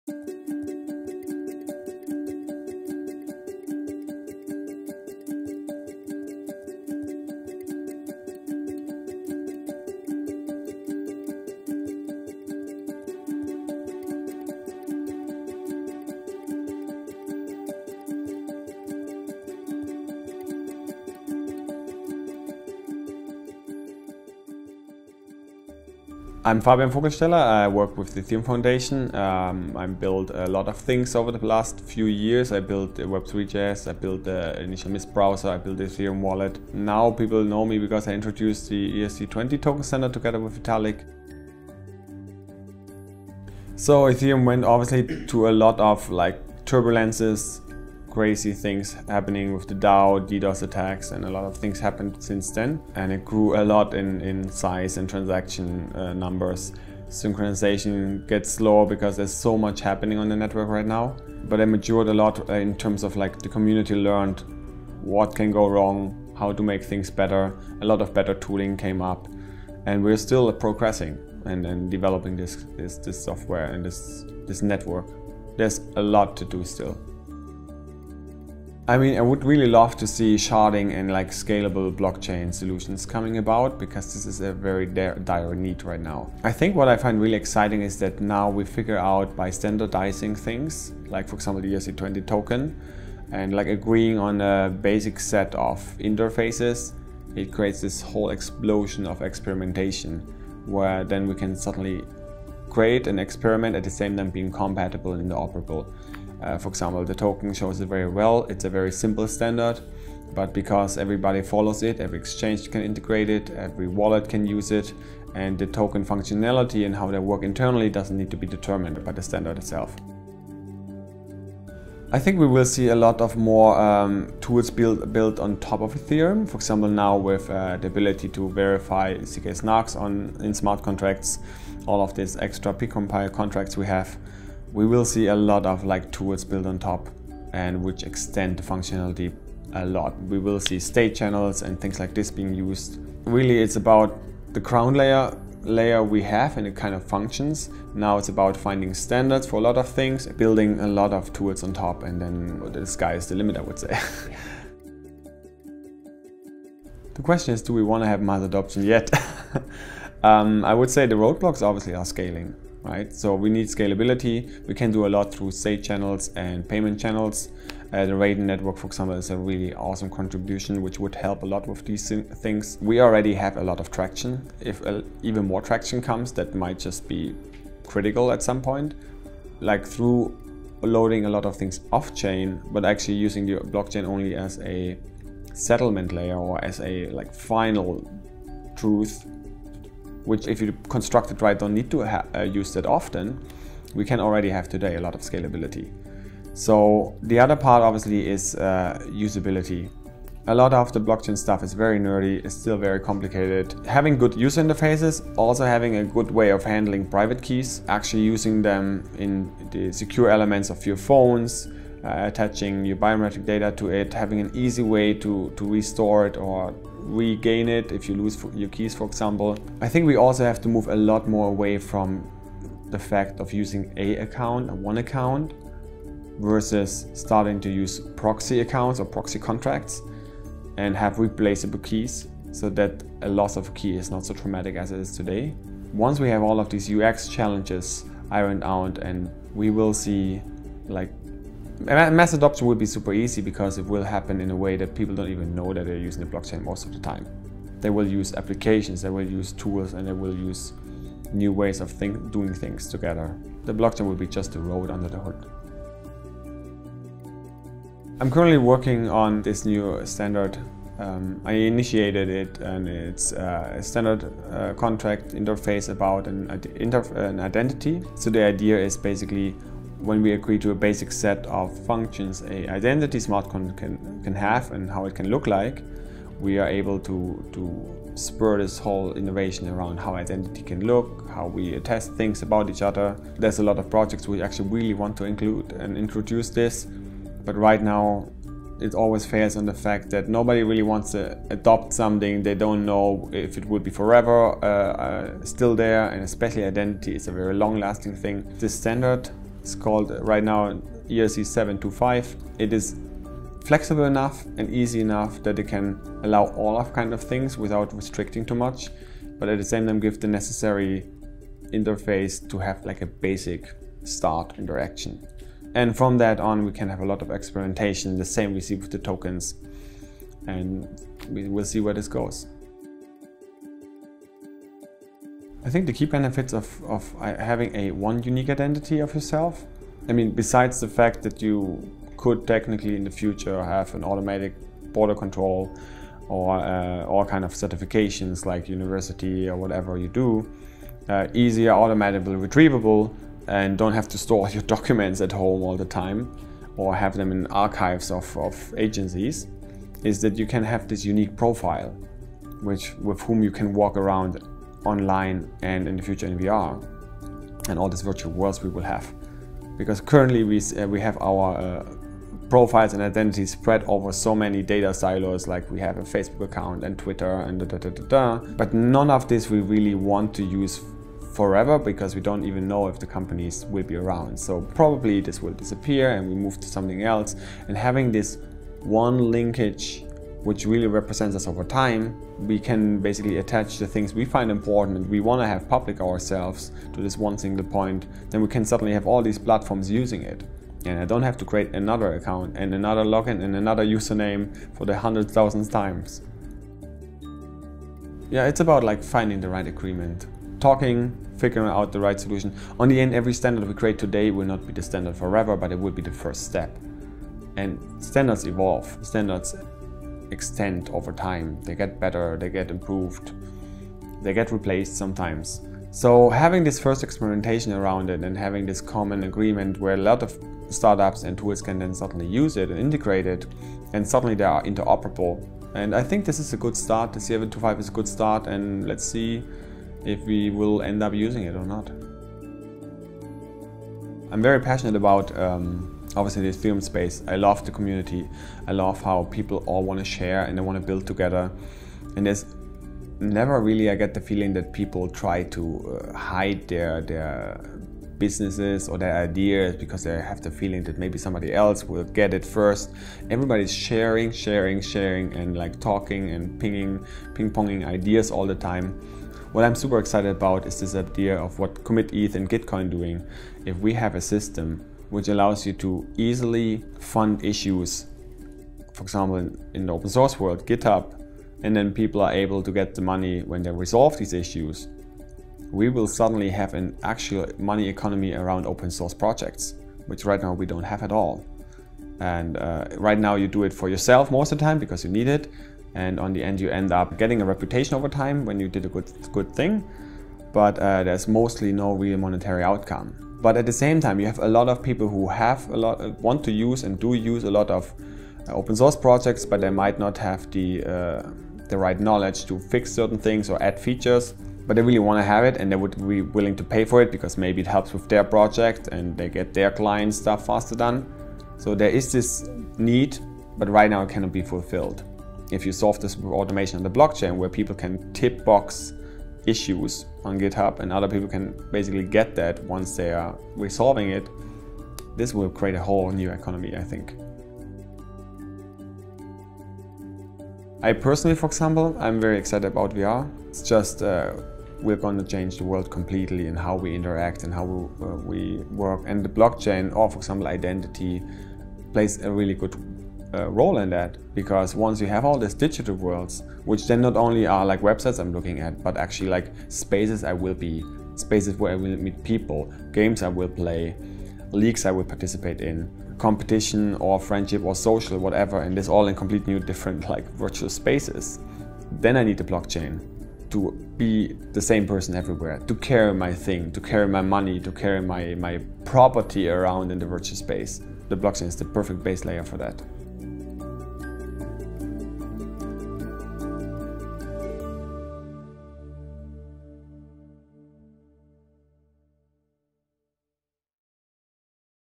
Fabian Vogelsteller // An Ethereum Interview Series
In round 2 of our Ethereum Interview Series, we caught up with Fabian Vogelsteller, Mist and web3.js developer with Ethereum. We discussed Ethereum, the progress made over the past year as well as the key challenges ahead. We also got learn more about his exciting work for an Ethereum-based identity standard - ERC725.